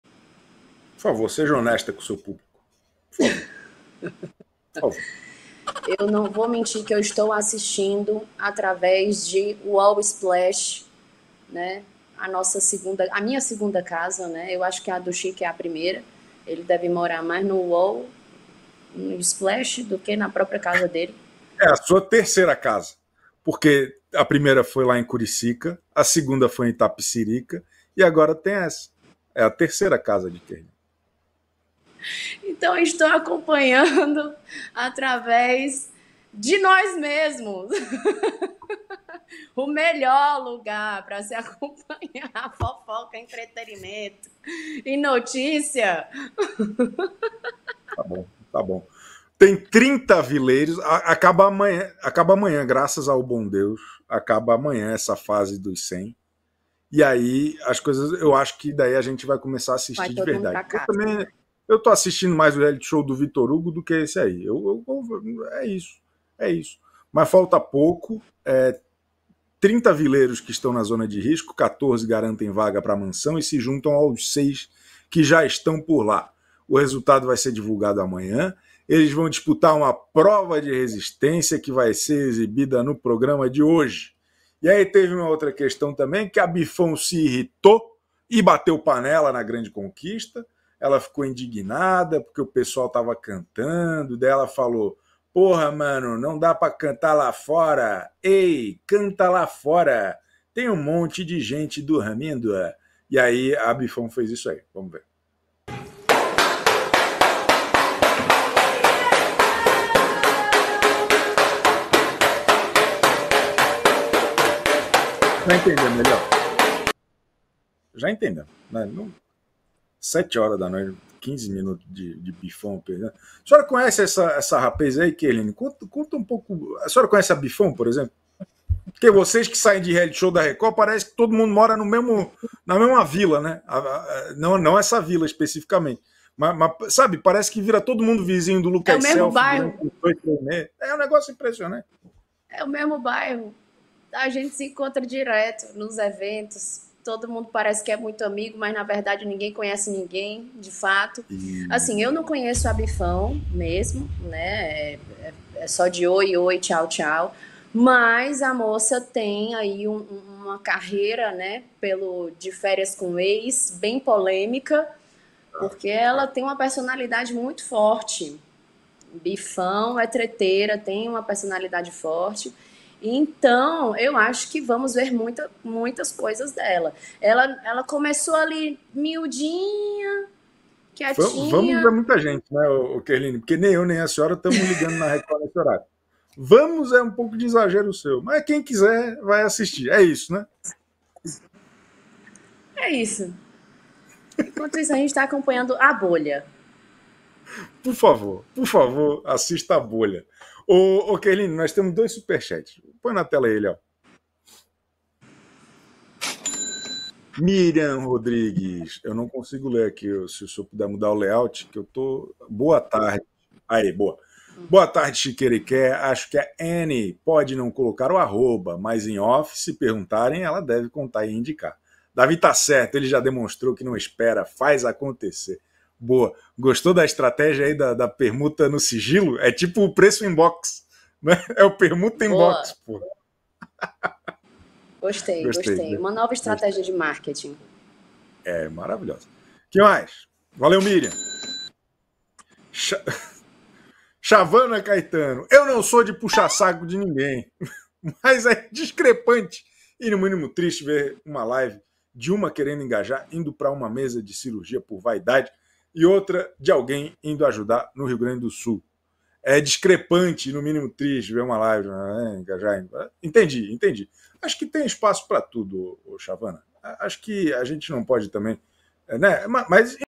De personalidade forte, Bifão não deve sumir tão cedo da TV, diz Kerline
No Central Splash de hoje, Chico Barney e Kerline comentam as últimas notícias dos ex-BBBs, famosos e A Grande Conquista!

Bem-vindos ao canal Splash, do UOL, onde você encontra nossa cobertura de celebridades, músicas, filmes, séries, reality shows e programas originais.